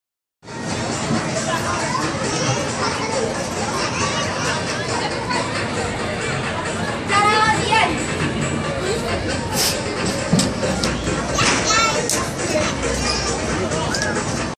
Miami Dade Fair 2015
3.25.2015 - Nana's first visit to the Miami Dade Fair & Expo. Having fun with Samantha.